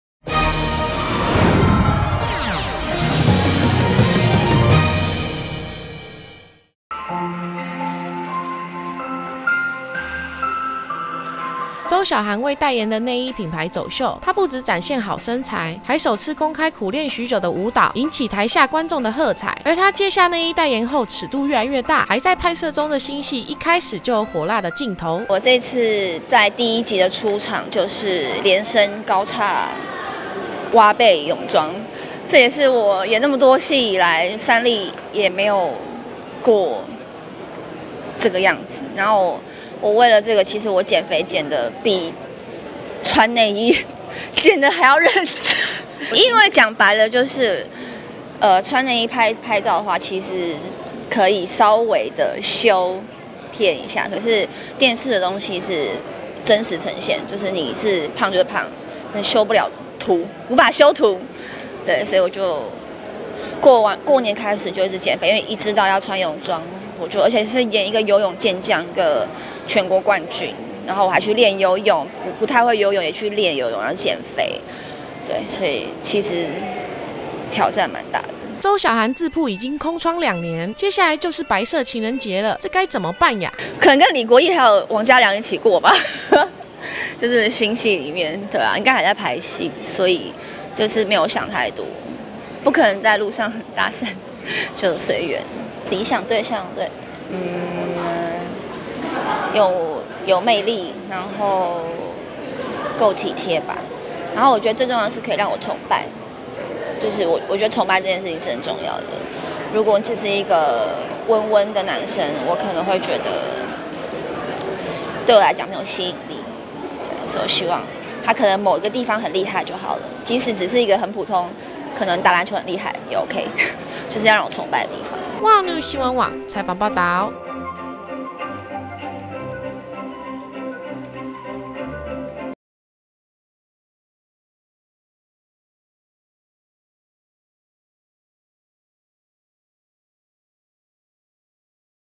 周曉涵內衣走秀展現輕熟女魅力 空窗兩年情人節和李國毅過(20160309)
周曉涵代言思薇爾內衣尺度大開，除了挑戰演技再進步之外，以往惜肉如金的她對裸露敬謝不敏，2016為突破自己不只是電眼美女，據傳新戲裡飾演一個游泳國手，比基尼上戲自然是少不了，只是演什麼要像什麼的周曉涵卻偏偏還學不會換氣，最近勤練泳技拒用替身，全部親自上陣；當然代言廠商一定是全力贊助戲裡需要的性感內衣，因為在市場不景氣的狀況下，周曉涵代言的內衣品牌2015年竟然開出紅盤，可以說是廠商的幸運星。

適逢思薇爾品牌30周年，今天發表會也以花都巴黎愛情海為主題，特別安排代言人周曉涵表演一段舞蹈，這對平日自認音感不足節奏不怎麼協調的她來說，比演戲還難；日前因拍戲不慎受傷，但敬業的她還說要來一段巴黎紅磨坊的「上空瘋馬秀」嚇壞經紀人。

不料，真正到了總彩階段，由於舞蹈老師設計的動作過大，周曉涵因腳傷不小心一滑，現場差一點上演「上空秀」，嚇得經紀人趕緊請老師改動作，還是乖乖地當愛情海女神，優雅地跳現代舞就好。

周曉涵這次代言拍攝廣告中化身「愛情海女神」，說也奇妙，自從扮演愛情海女神之後，身邊的姐妹淘在2月14日情人節當天，全部都被示愛，甚至決定結婚，紛紛認為是沾了周曉涵的幸運，讓始終單身的周曉涵傻眼；決定拒絕再當姐妹淘的愛神紅娘，把好運留給自己，但忙於拍戲的她又苦無機會，只好主動大聲呼喊愛情，催促身邊對她有意思的人盡量主動示愛，最好在3月14日白色情人節前，別讓她的愛情進度輸姐妹淘太多。